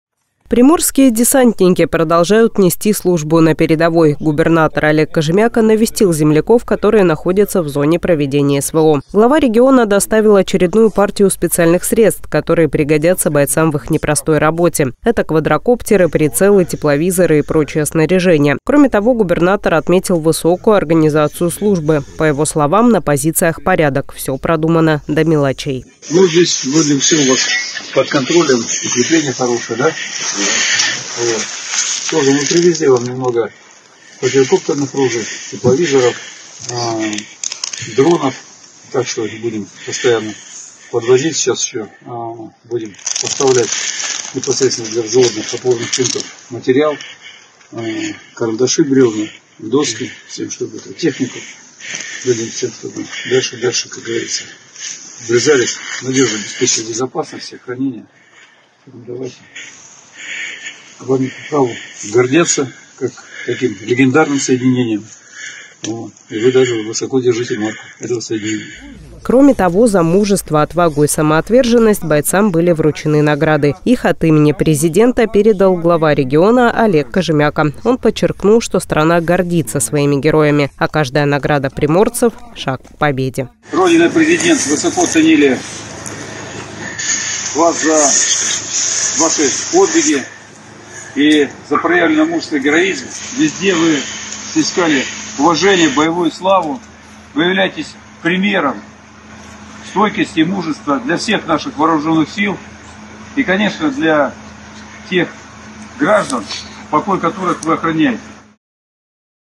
Губернатор Приморья доставил бойцам спецсредства на передовую в зону СВО.
Приморские десантники продолжают нести службу на передовой. Губернатор Олег Кожемяко навестил земляков, которые находятся в зоне проведения СВО. Глава региона доставил очередную партию специальных средств, которые пригодятся бойцам в их непростой работе. Это квадрокоптеры, прицелы, тепловизоры и прочее снаряжение. Кроме того, губернатор отметил высокую организацию службы. По его словам, на позициях порядок, всё продумано до мелочей.